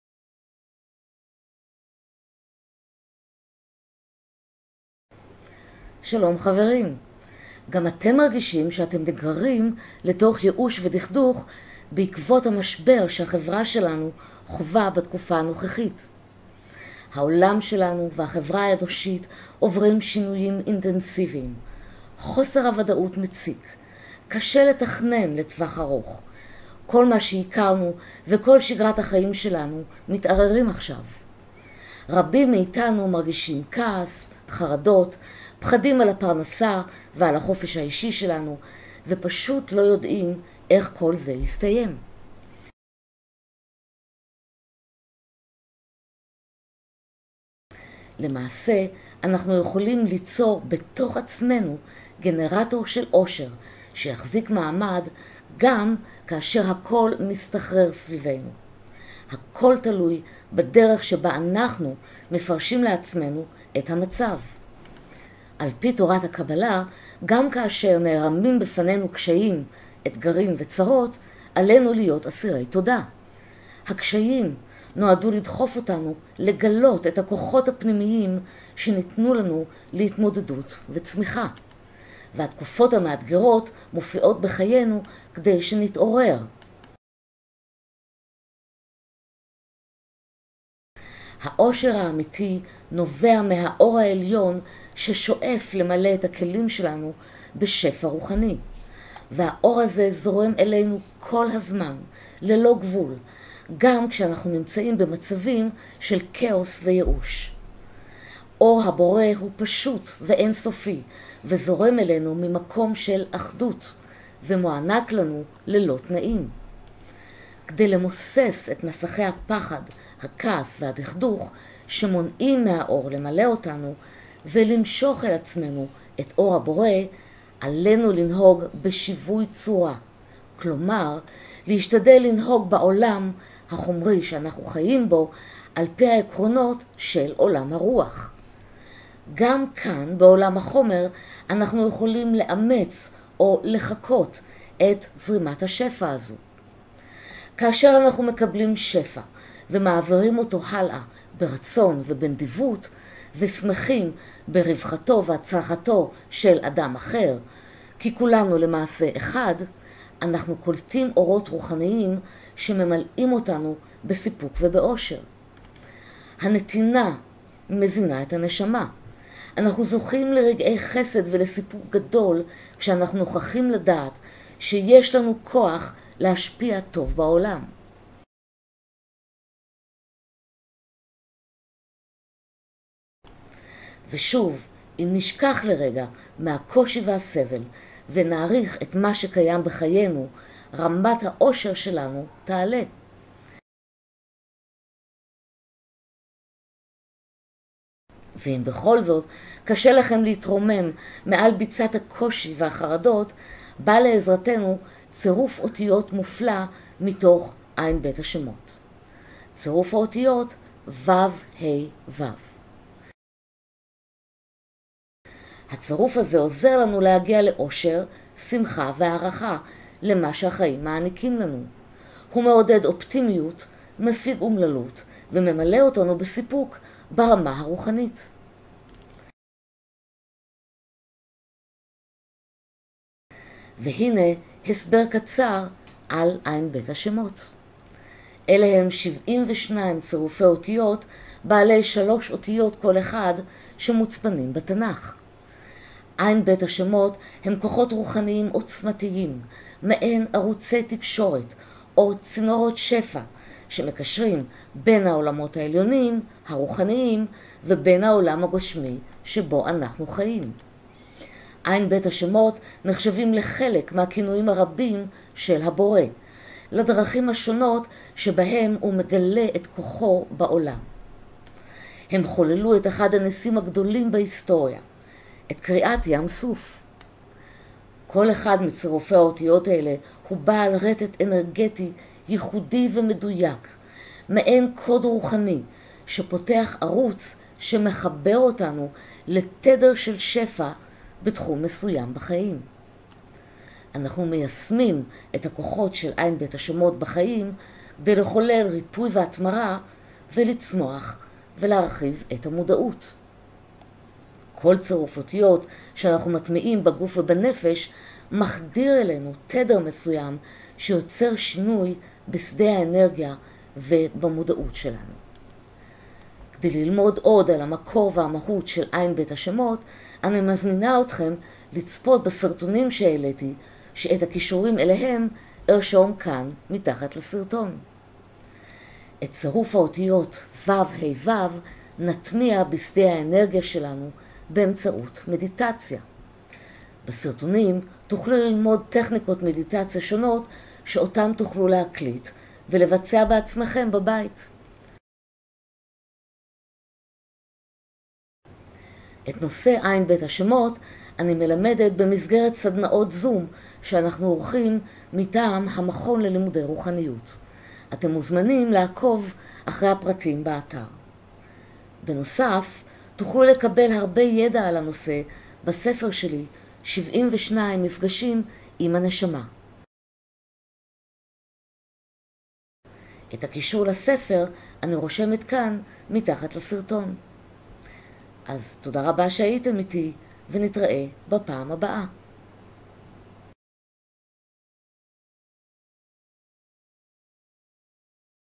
איך לשמור על האושר הפרטי שלנו בזמנים קשים ? צירוף האותיות ו.ה.ו מתוך ע"ב השמות
העולם שלנו והחברה האנושית עוברים שינויים אינטנסיביים, חוסר הוודאות מציק, קשה לתכנן לטווח ארוך, כל מה שהכרנו, וכל שגרת החיים שלנו מתערערים עכשיו...
רבים מאיתנו מרגישים כעס, חרדות, פוחדים על הפרנסה ועל החופש האישי שלנו, ופשוט לא יודעים איך כל זה יסתיים.
איך אפשר לשמור על איזון ועמוד שדרה פנימי מואר, או להיות מאושרים בתקופות קשות?
ואיך יכולים לעזור לנו ע"ב השמות?
צפו בשיעור קצר המציג את דרך פעולתו של צירוף האותיות ו.ה.ו .
כדי ללמוד עוד על המקור והמהות של ע"ב השמות, אני מזמינה אתכם לצפות בסרטונים שהעליתי, הכוללים טכניקות מדיטציה והטמעה.